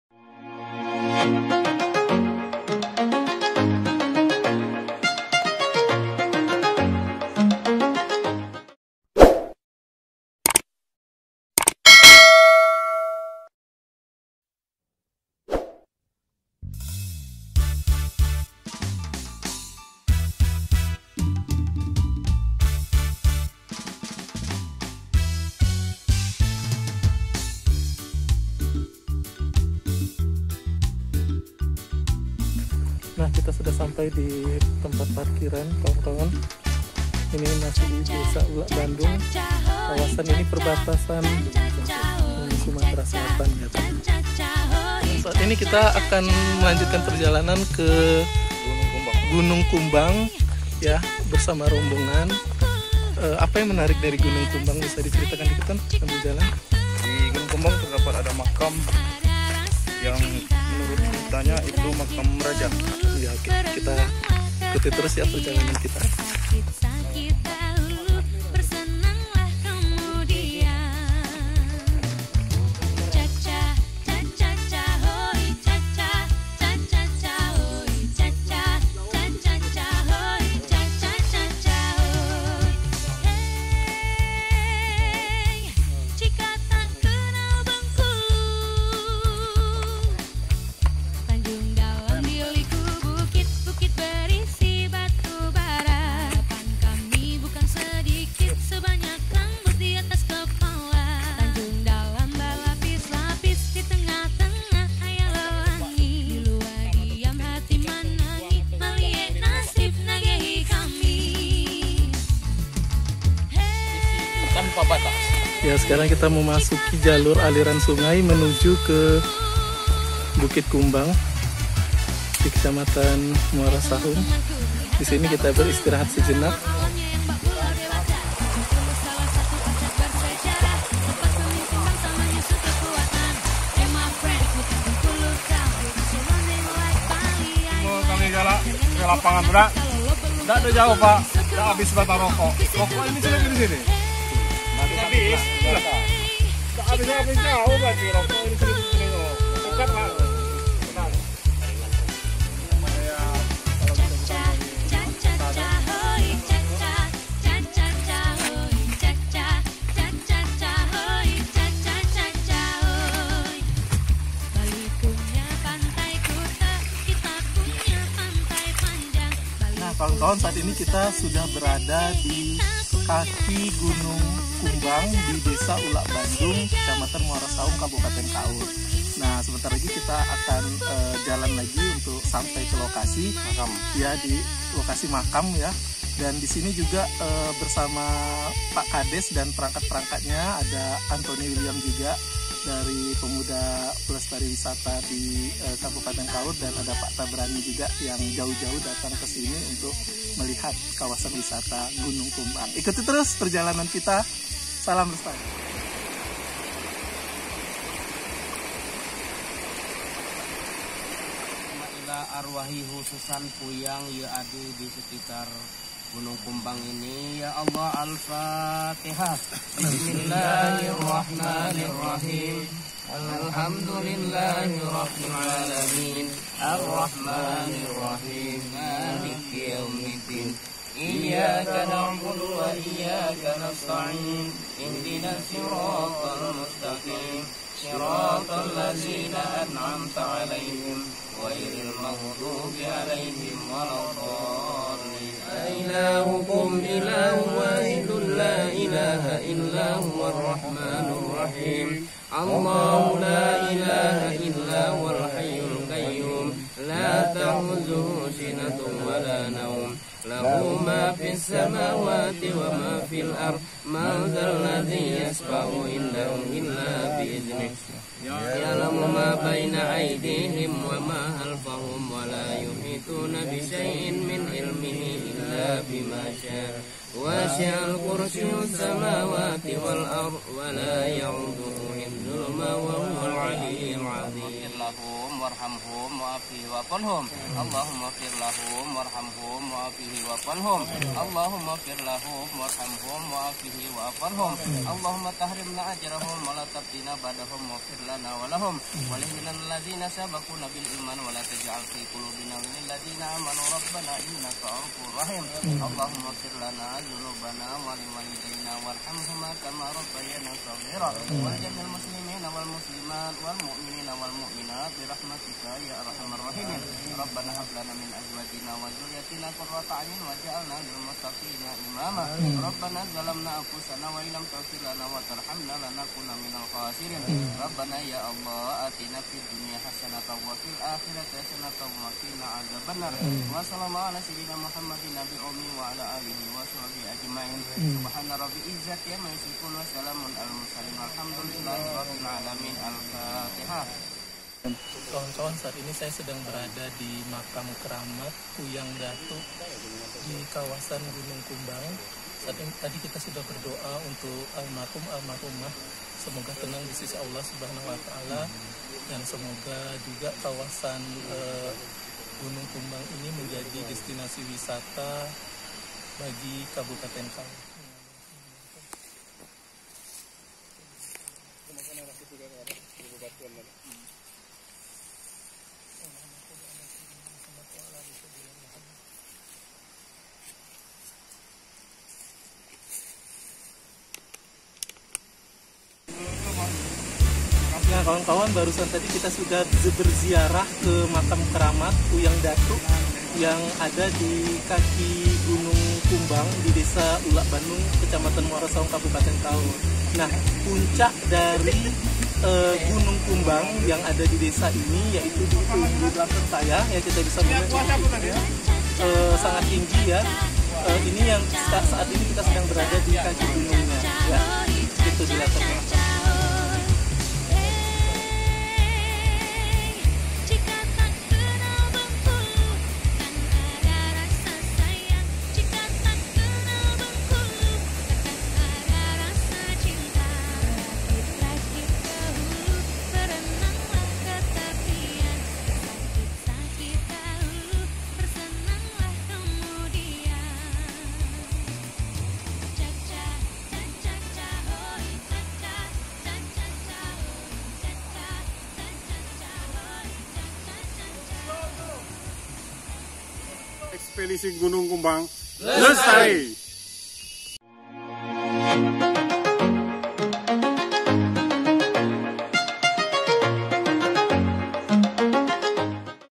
Ziarah Ke Bukit Kumbang, Bukit Penuh Misteri di Kabupaten Kaur
Ekspedisi Bukit Kumbang

Bukit Kumbang berlokasi  di perbatasan antara Kabupaten Kaur dan Provinsi Sumatera Selatan dimana untuk sampai ke lokasi ini memerlukan waktu  sekitar 2 jam tracking berjalan kaki menyusuri kawasan hutan rimba yang masih sangat asri.

Selama perjalan kami menjumpai tumbuhan liar seperti Pasak bumi, Caladium, Monstera, Calatea,  Amorphophallus titanum, Anggrek tanah dan beberapa tumbuhan kayu, seperti kayu besi, kayu samurai, dan rotan. Tidak dijumpai bunga Rafflesia dilokasi ini walau ada beberapa tanaman yang mirip dengan Tetrastigma (tanaman inang bunga Rafflesia).

Pemandangan alam ketika berada di kaki bukit kumbang sungguh indah, Terdapat sungai yang diapit dinding batu raksasa yang begitu unik, dan  juga makam keramat yang berada di pinggiran sungai.

Terimakasih kepada bro @antonywiliamwiliam, Pak Kades Desa Ulak Bandung beserta perangkatnya yg sudah memandu kami ke lokasi ini

Juga terima kasih kepada bapak @thabranisakila yg sdh mengajak saya @sofianrafflesia  dan @dodhyshaputra untuk ikut dalam  ekspedisi 1 hari mengunjungi 5 spot wisata yg sangat luar biasa di desa Ulak Bandung kecamatan Muara Sahung Kabupaten Kaur.